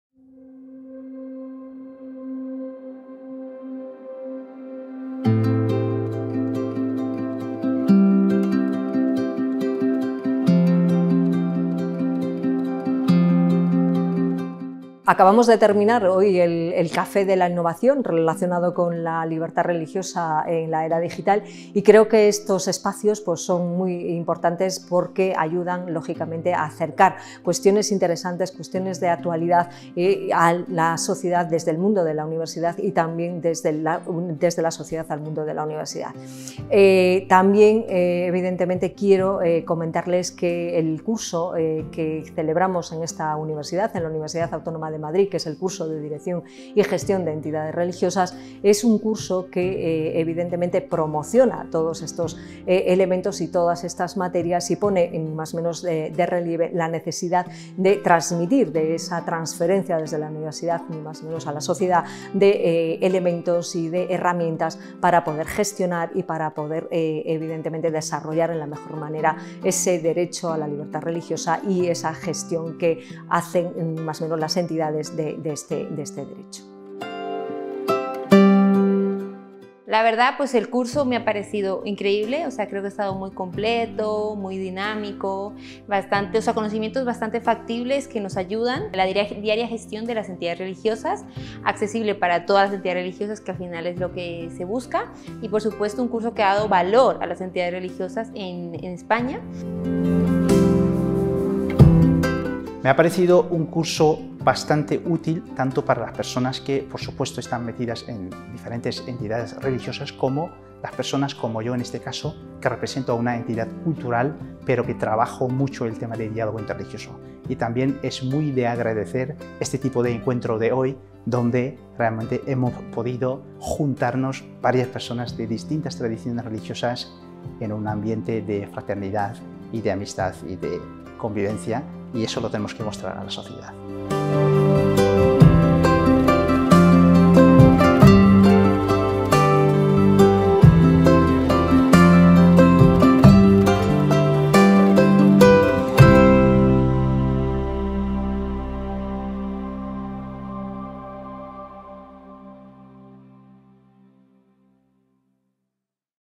Resumen CaféUAM El derecho de la libertad religiosa en la era digital
El surgimiento de las nuevas tecnologías en la sociedad contemporánea ha obligado a actualizar el derecho desde múltiples puntos de vista y la libertad religiosa no es ajena a este fenómeno. Particularmente, la irrupción del factor internet, las TIC, el Big Data y especialmente, las inteligencias artificiales, ha implicado que la Unión Europea y los distintos Legisladores de los Estados miembros, deban acometer ciertas reformas urgentes en sus Ordenamientos jurídicos, de modo que se puedan regular determinados temas éticos, fiscales, laborales, civiles, penales, etc, con una especial consideración en materia de protección de datos o sobre de cumplimiento normativo.

Precisamente, la Unión Europea solicitaba por ello ampliar el concepto de personalidad jurídica de modo que fuese aplicable a las inteligencias artificiales, habida cuenta de sus peculiaridades y especificidades. Para poder acceder a estas iniciativas, de carácter jurídico multidisciplinar y transversal, no se puede prescindir del Derecho Eclesiástico del Estado ni del Derecho de las diferentes confesiones religiosas.